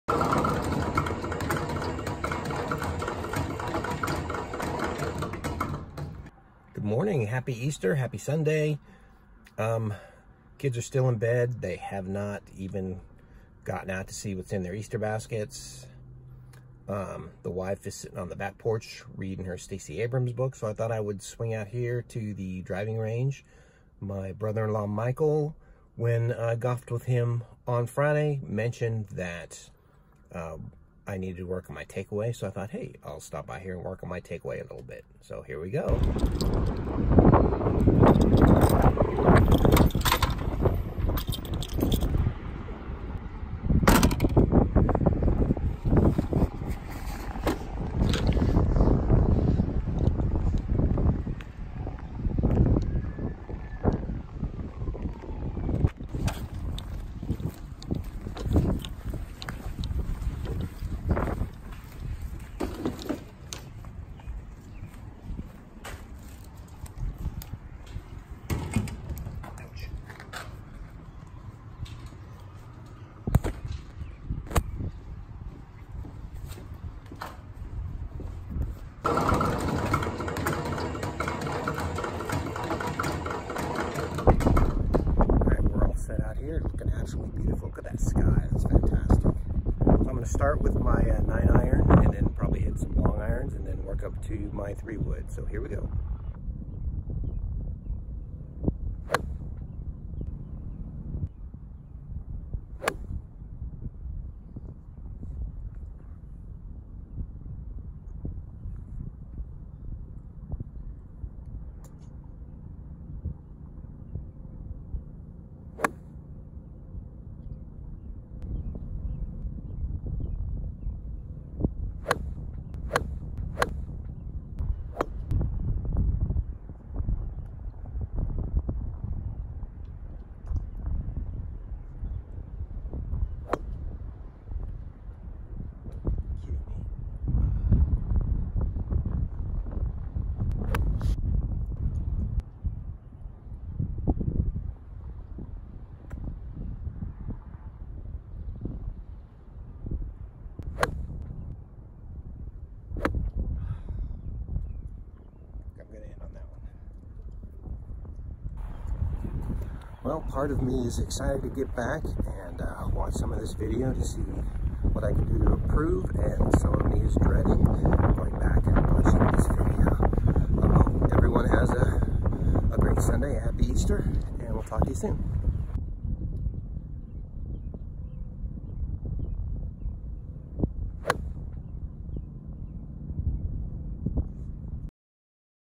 Vlog 190421
Just in case you needed to see an old man hack at balls for three minutes...